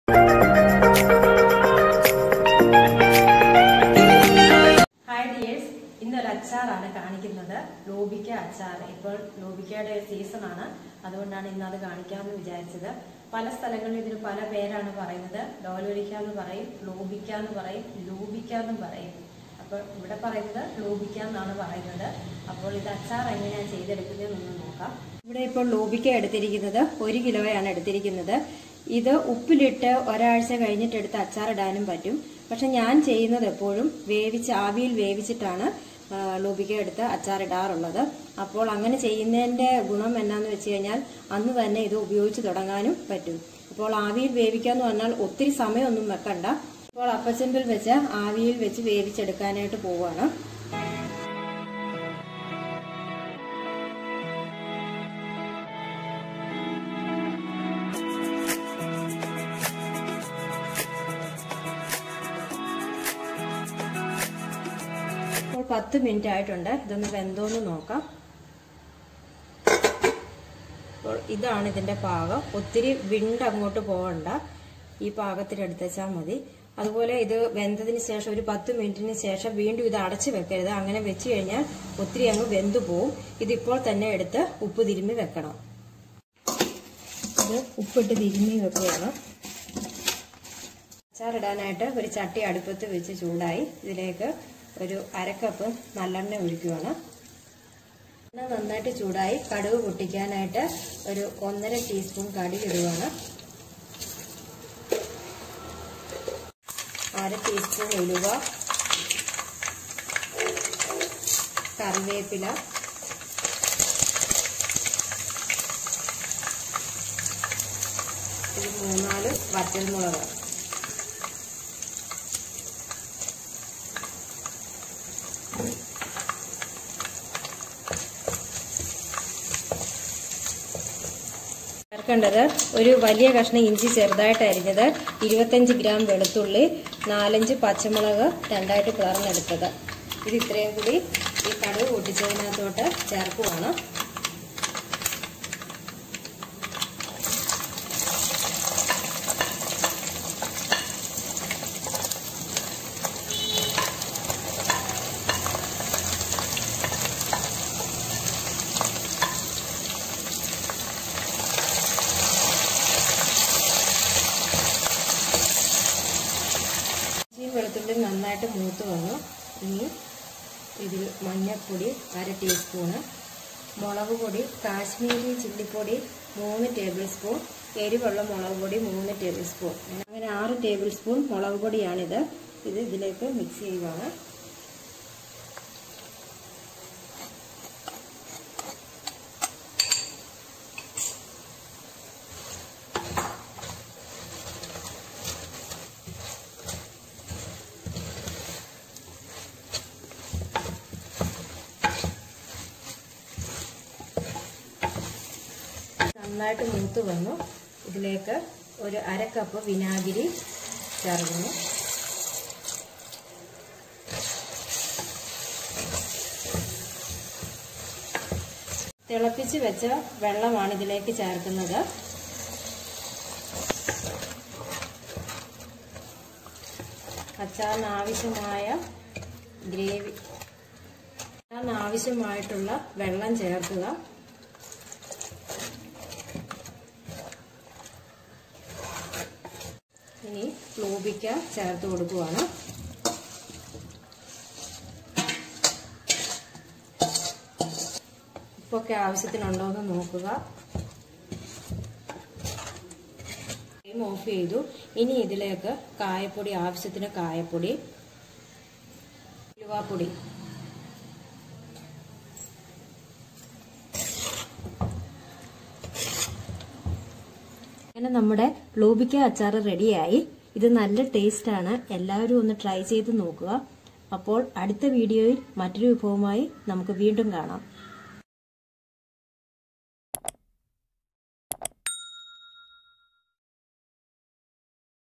Loobikka Achar||ലൂബിക്ക അച്ചാർ||Pickle Recipes
Hello dears😍please leave your valuable comments about this video.

Ingredients :
Loopikka/ലൂബിക്ക  -1kg
Chilly powder/മുളക് പൊടി -6 tbs
Nallenna/നല്ലെണ്ണ -1/2 cup
Fenugreek Powder/ഉലുവ പൊടി -1/4 tbs
Kayapodi/കായപൊടി -1 tbs
Ginger/ഇഞ്ചി - medium size
Garlic/വെളുത്തുള്ളി -25g
Salt/ഉപ്പ്
Mustard Seeds/കടുക്
Fenugreek /ഉലുവ
Curryleaves /കറുവേപ്പില
Dry Red Chillies /വറ്റൽമുളക്